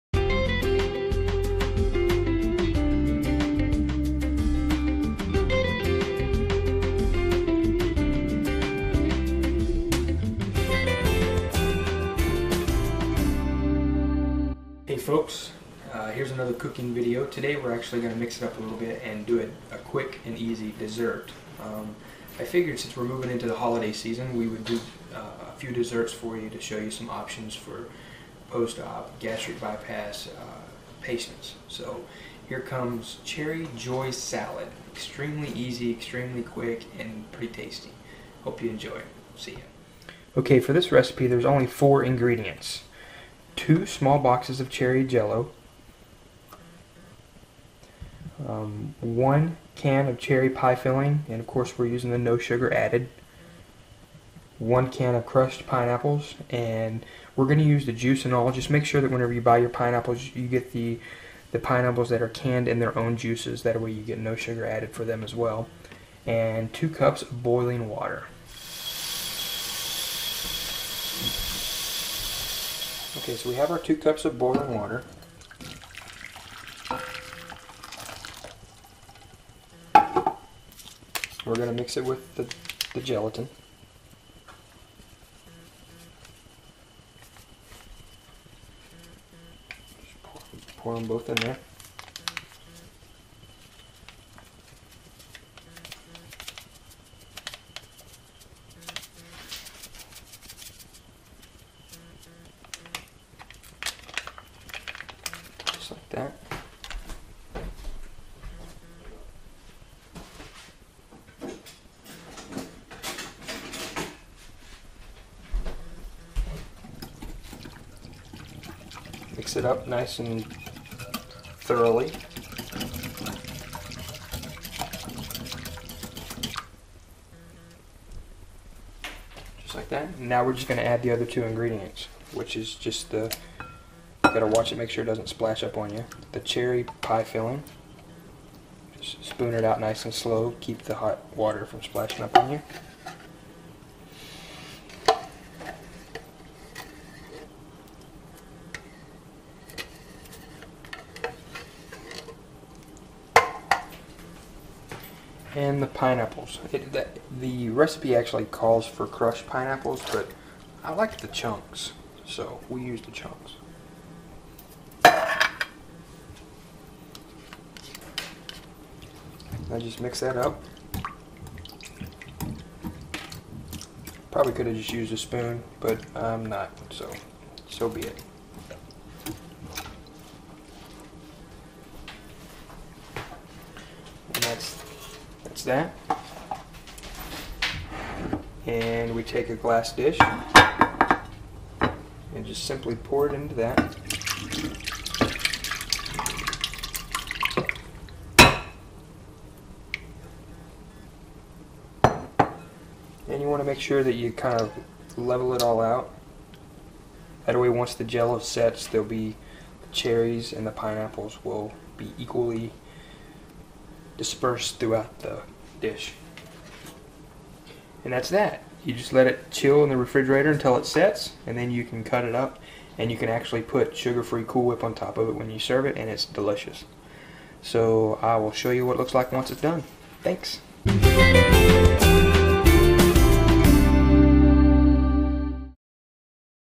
Cherry Joy Salad - Cooking after Gastric Bypass
Cherry Joy Salad

2 small boxes cherry gelatin (sugar free)
2 cup hot boiling water
1 can cherry pie filling, light
1 can crushed pineapple, juice and all

Combine gelatin and boiling water. Add remaining ingredients. Pour in a 9 x 13-inch glass baking dish.

Top with Cool Whip (No Sugar Added) when serving.